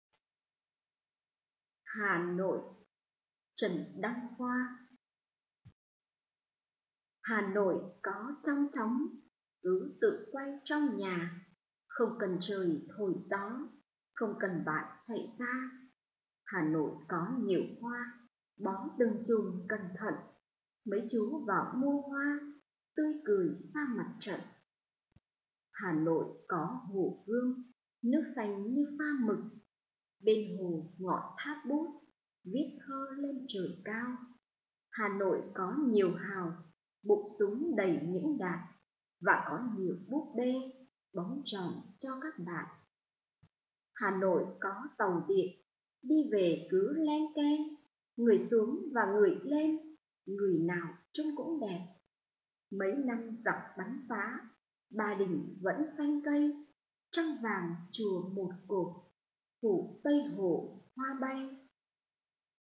Bài thơ: HÀ NỘI (Trần Đăng Khoa)
HÀ NỘI (Trần Đăng Khoa)

Hà Nội có chong chóng
Cứ tự quay trong nhà
Không cần trời thổi gió
Không cần bạn chạy xa

Hà Nội có nhiều hoa
Bó từng chùm cẩn thận
Mấy chú vào mua hoa
Tươi cười ra mặt trận

Hà Nội có Hồ Gươm
Nước xanh như pha mực
Bên hồ ngọn Tháp Bút
Viết thơ lên trời cao

Hà Nội có nhiều hào
Bụng súng đầy những đạn
Và có nhiều búp bê
Bóng tròn cho các bạn

Hà Nội có tàu điện
Đi về cứ leng keng
Người xuống và người lên
Người nào trông cũng đẹp

Mấy năm giặc bắn phá
Ba Đình vẫn xanh cây
Trăng vàng chùa Một Cột
Phủ Tây Hồ hoa bay...

1969